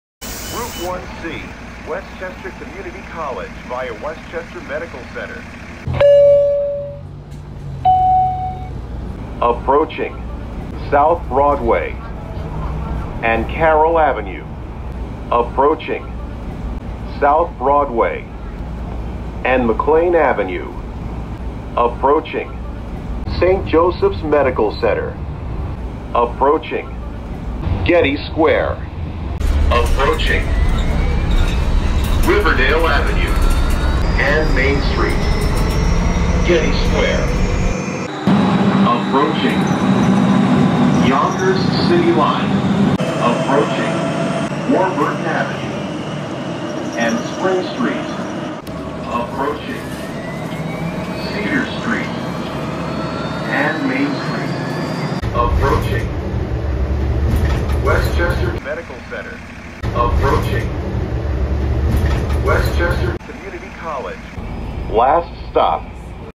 The Bee-Line System || W1C Bus Announcements To WCC Via WMC || Via Saw Mill River Road
Credit: @Gueli844 for some of the announcements, Some of them were recorded by me